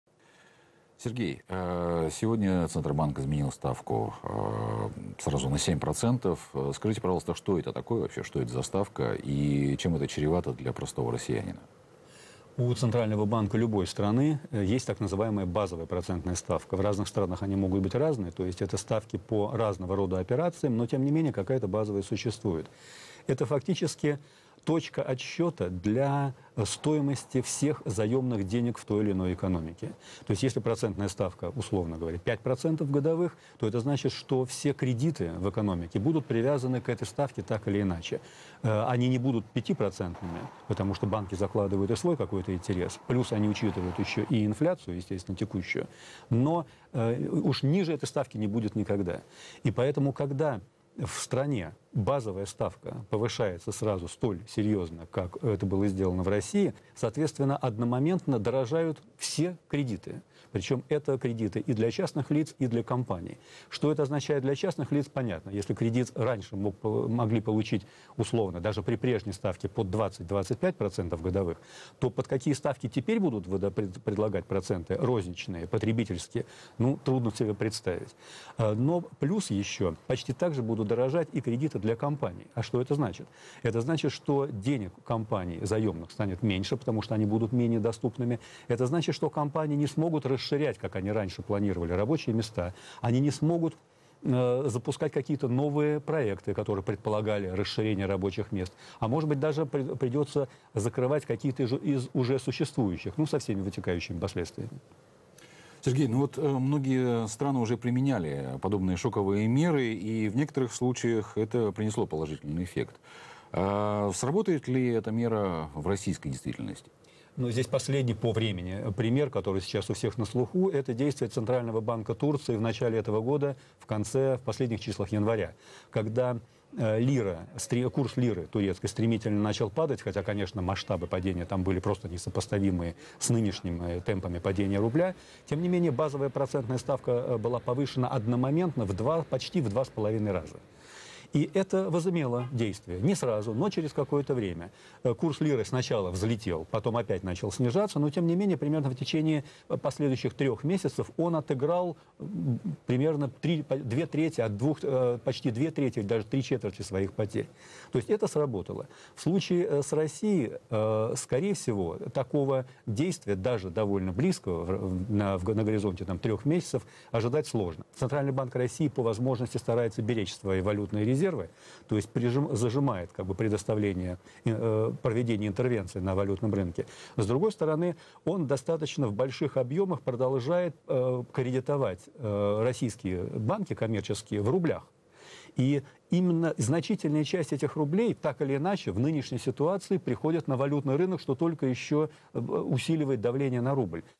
Крах рубля
Сергей Сенинский, экономический обозреватель “Радио Свобода”, о шоковых мерах по спасению курса рубля